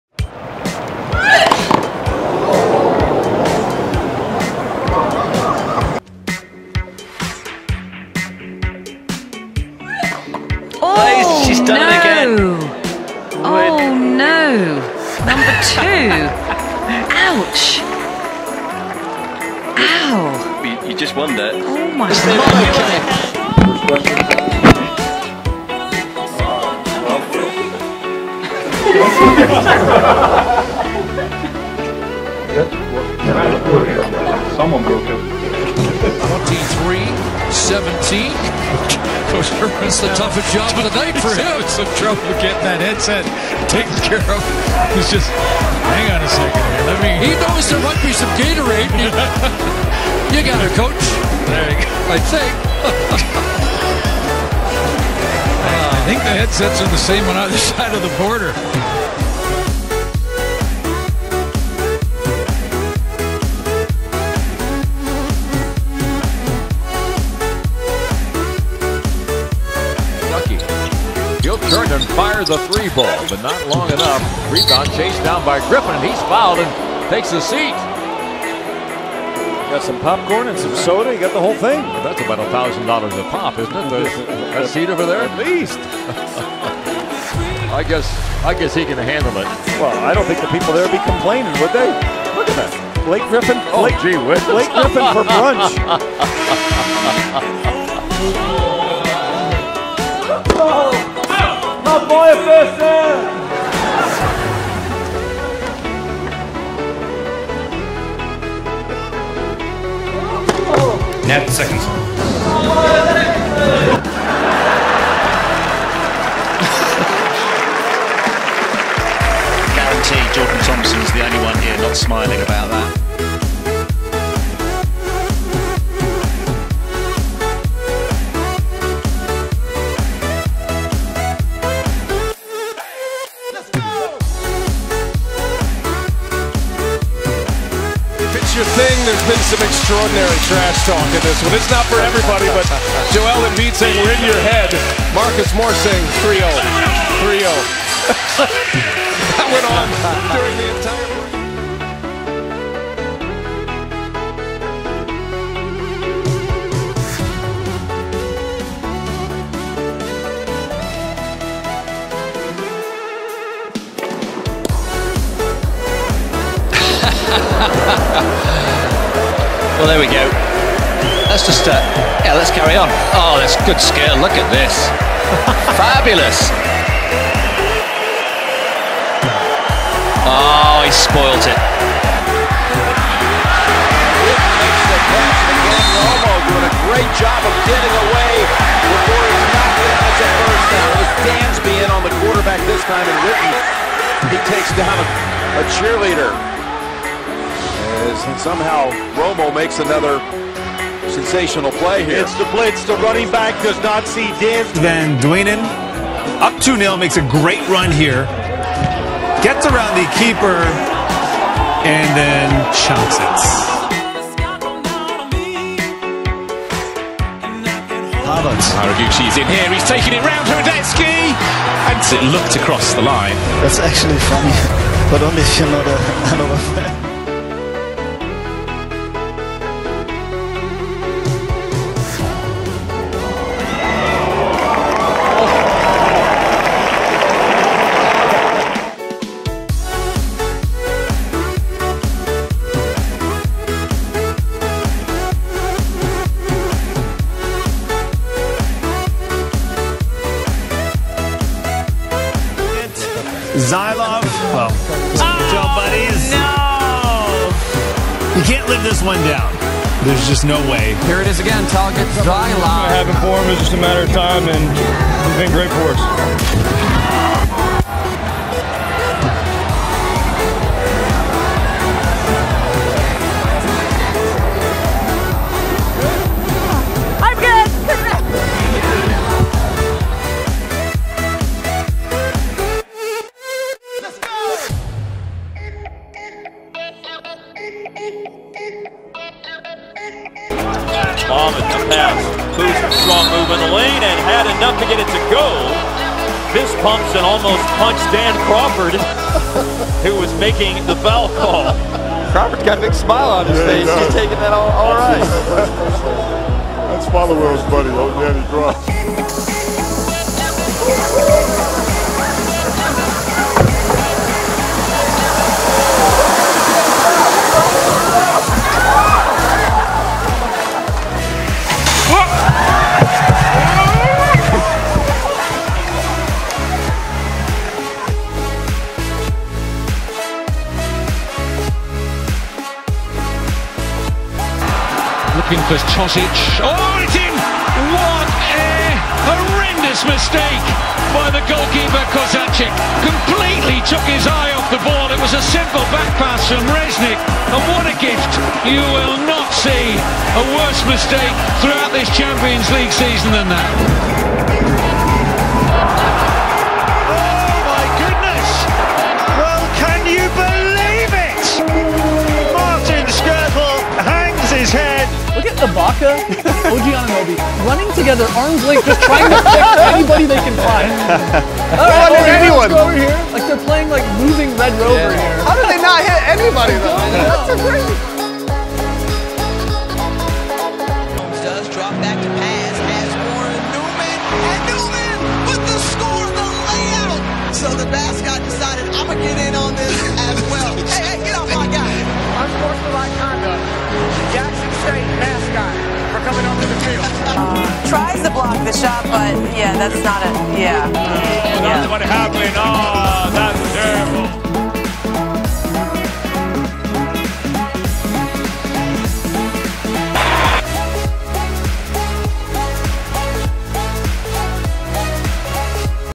Most Funny Moments in Sports #3
Most Funny Moments in Sports #3 video with you. There are many star names in the video, such as Bianca Andreescu, Naomi Osaka, Nadal, Jokic and Joel Embiid.